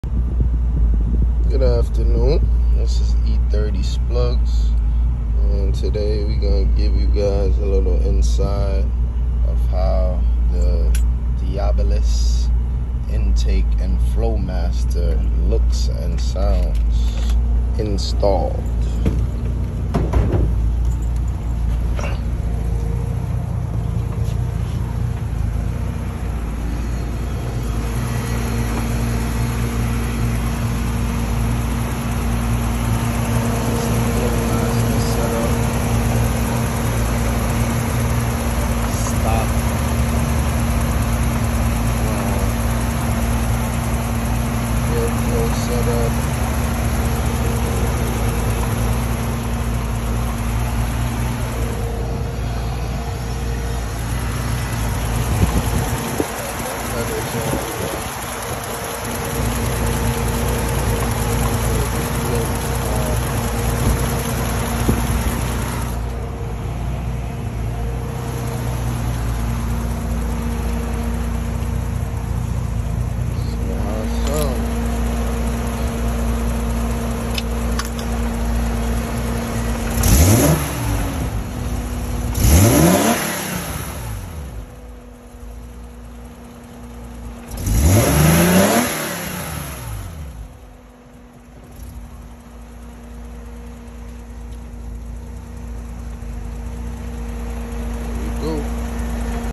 Dbilas Dynamic itbs & flowmaster intake with performance chip completely stock #885 m20 engine.
Video of splugz on his e30 performance journey naturally aspirated motor no turbocharged just all natural sounds of a bmw m20.Still fun and powerful